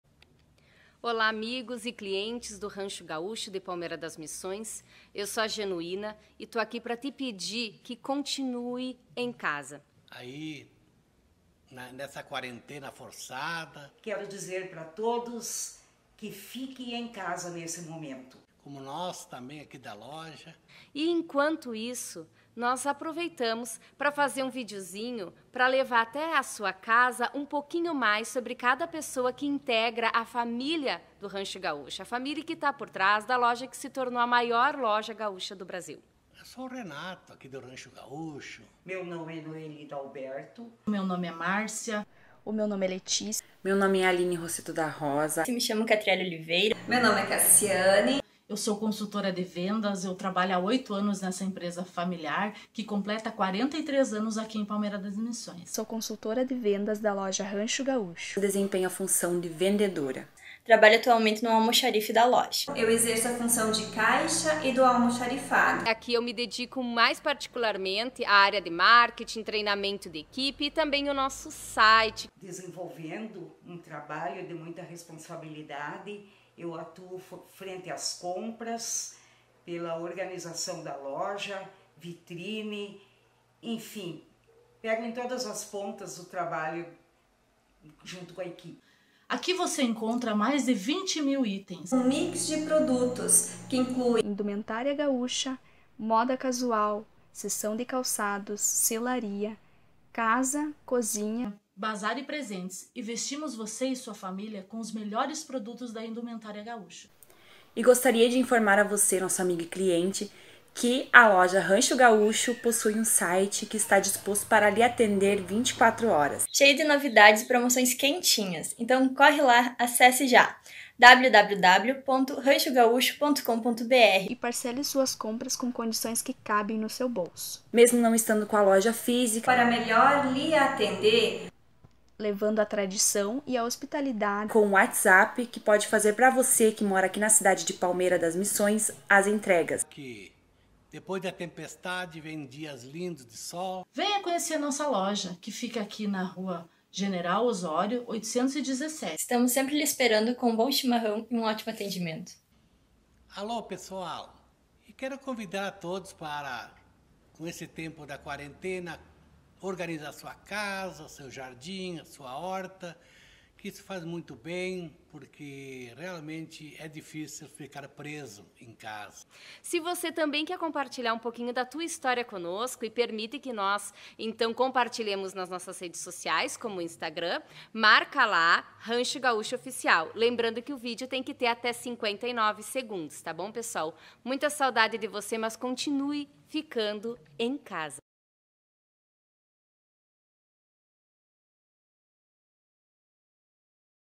Fique em Casa - Campanha Loja Rancho Gaúcho
Conheça proprietários e colaboradores da loja Rancho Gaúcho de Palmeira das Missões que os convidam para Ficar em Casa durante a Pandemia do COVID-19. ou via WhatsApp